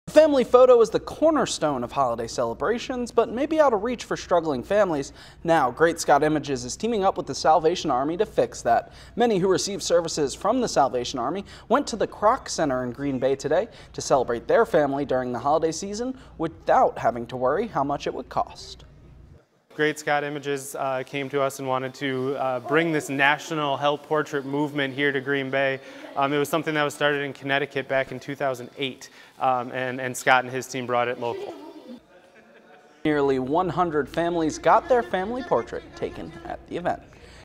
Salvation Army helps families take holiday portraits
A family photo is a cornerstone of holiday celebrations, but may be out of reach for struggling families.